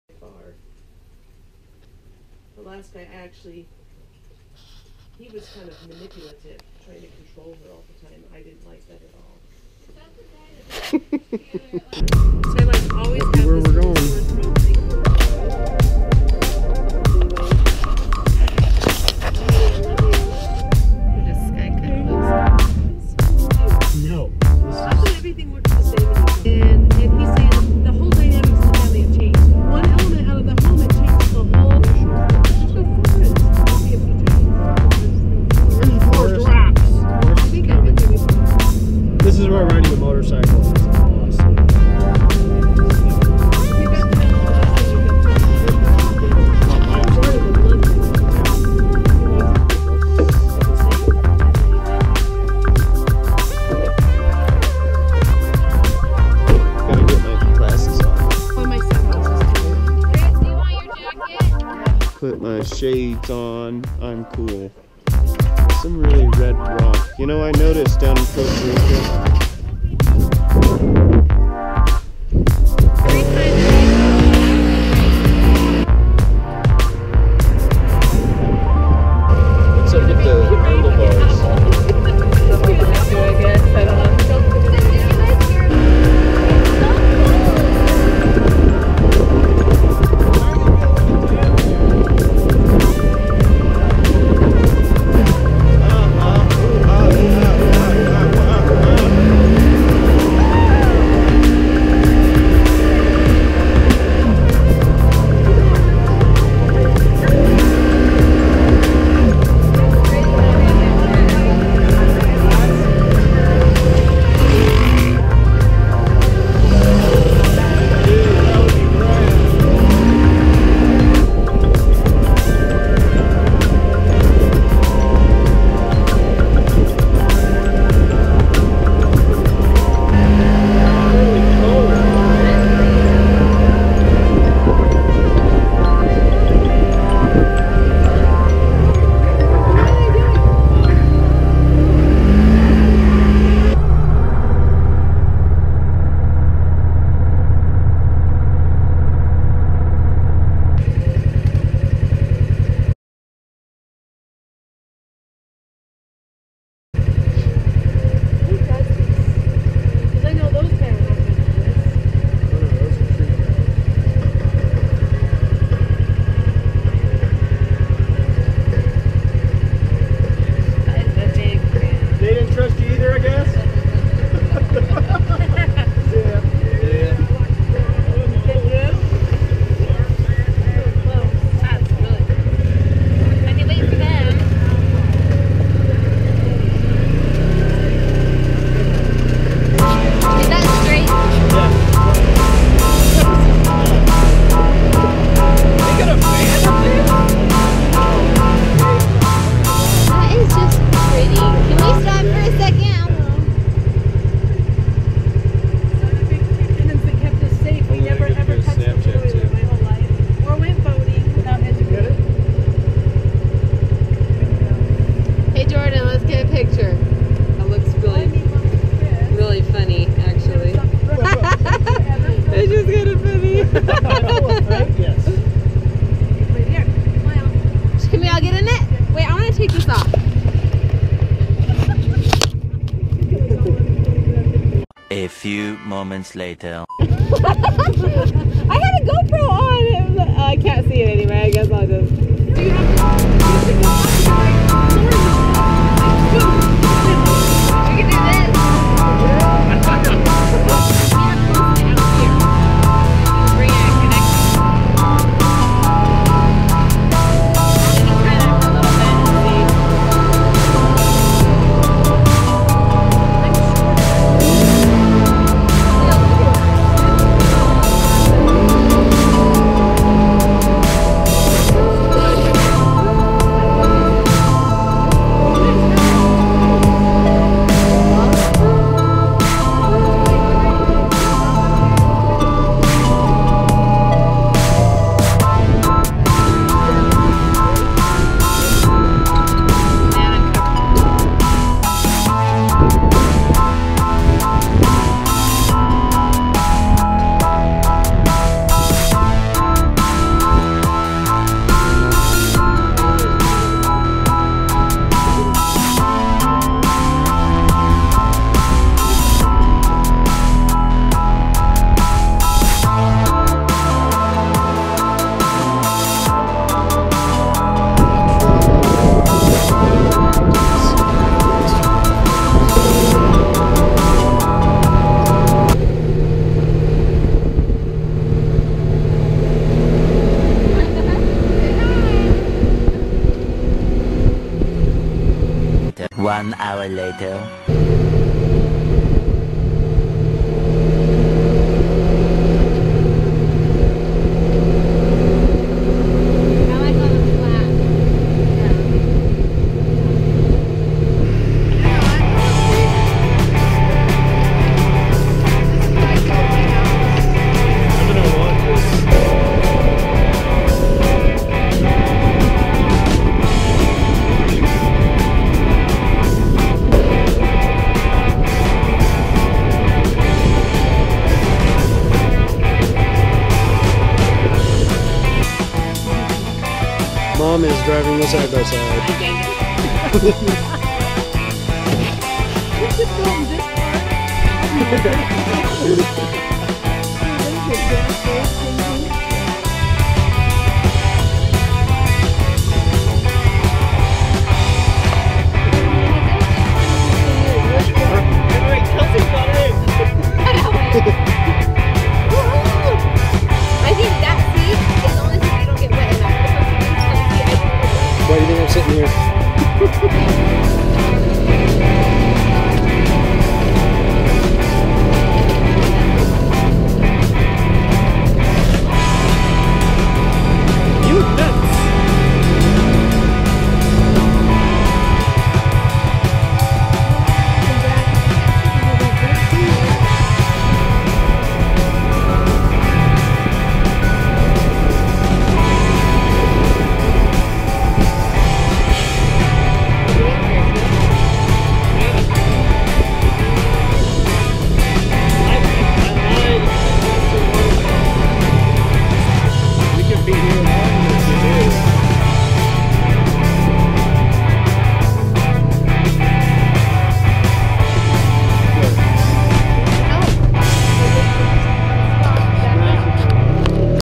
American Fork Ride for my Birthday!
This video is about American Fork Ride with Mom and Jordan on my birthday